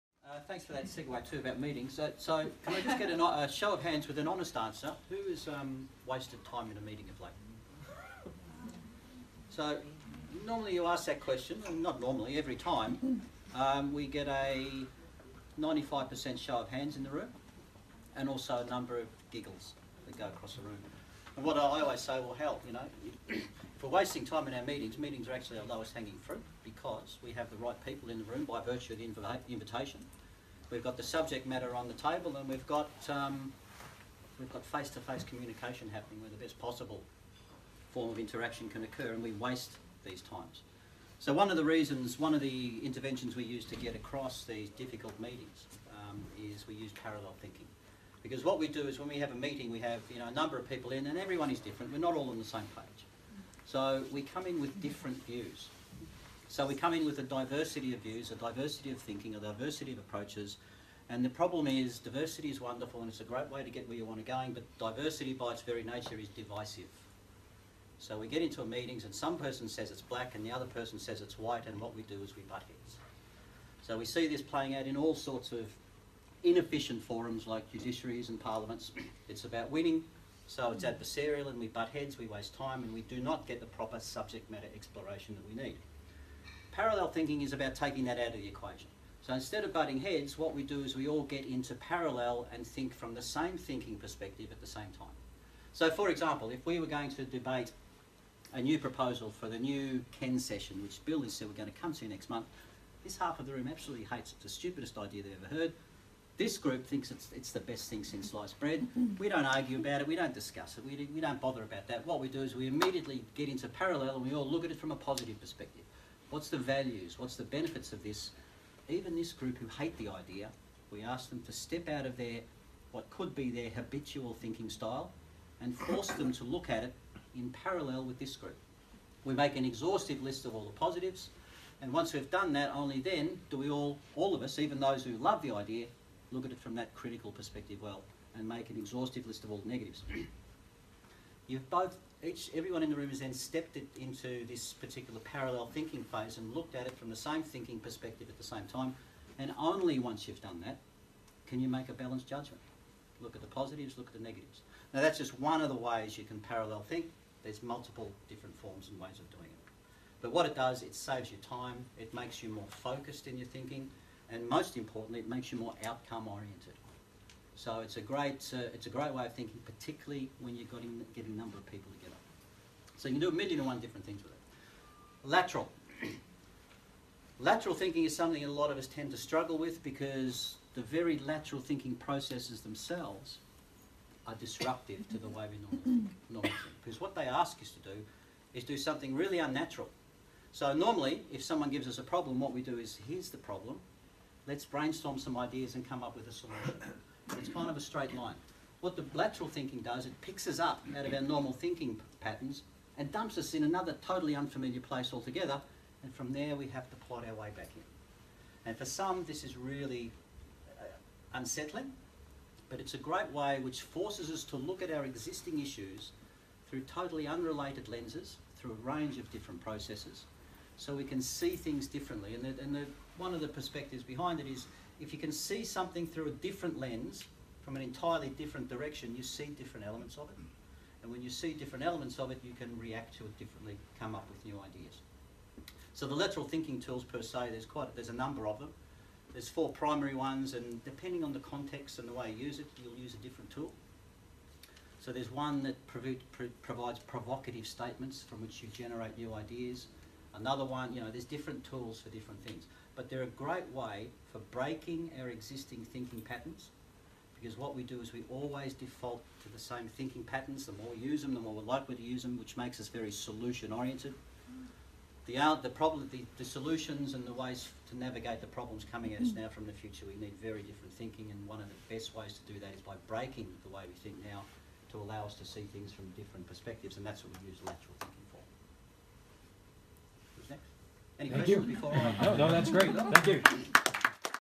Frank Connolly - Parallel & Lateral Thinking
Frank Connolly at the Mindwerx sponsored November 2016 Creativity Exchange Network event, presenting on Parallel and Lateral Thinking.